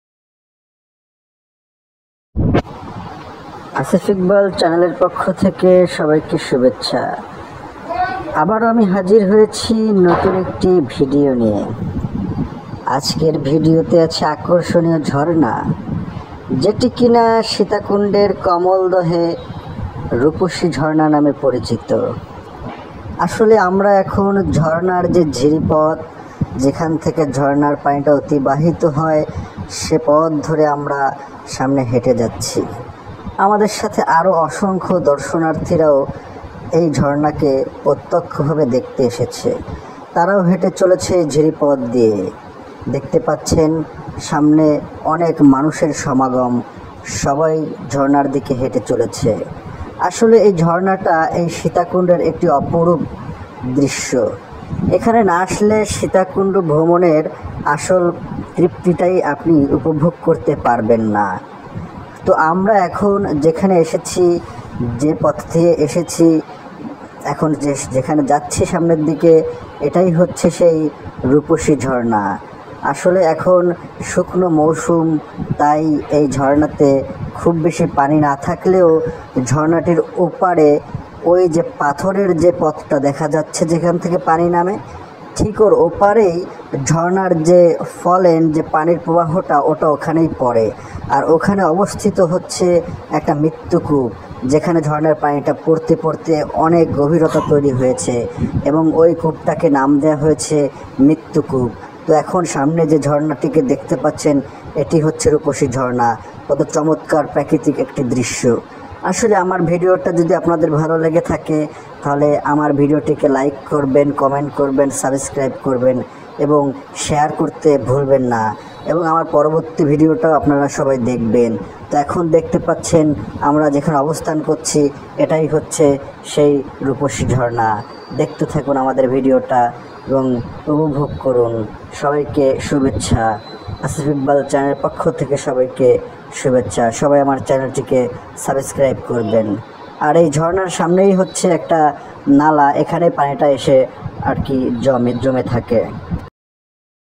রূপসী ঝর্ণা চট্টগ্রামের মিরসরাই সীতাকুন্ড || স্বচ্ছ পানির শীতল জলধারা || পাহাড়ঘেরা সুবজায়নের রূপ
বর্ষায় চট্টগ্রামের পাহাড়ঘেরা সুবজায়নের রূপ-প্রকৃতির সৌন্দর্য বিমোহিত করে প্রকৃতিপ্রেমীদের। নিভু নিভু করে চলতে থাকা পাহাড়ি ঝর্ণাধারা বর্ষার আগমনে প্রাণের সজিবতা ফিরে পেয়ে হাতছানি দিয়ে ঢাকতে থাকে ভ্রমণপিপাসুদের। তেমনি একটি অনিন্দ সুন্দর ভ্রমণ স্থান হলো বড় কমলদহ রূপসী ঝর্ণা। এই ঝর্ণাটির মনোমুগ্ধকর আকর্ষণীয় রূপের কারণে

স্থানীয়রা একে রূপসী ঝর্ণা নাম দিয়েছে। যা চট্টগ্রামের মিরসরাই উপজেলার ওয়াহিদপুর ইউনিয়নে বড়কমল এলাকায় অবস্থিত।